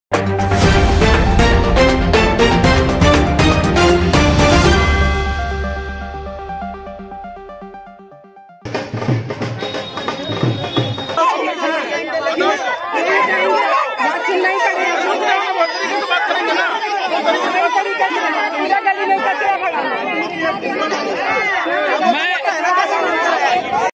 VBN - NEWS के रिपोर्टर से की बदसलूकी - कैमरामैन से की धक्का-मुक्की रितु राज झा विधायक AAP
VBN - NEWS वंदे भारत न्यूज़ के रिपोर्टर से की बदसलूकी -  कैमरामैन से की धक्का-मुक्की रितु राज झा विधायक AAP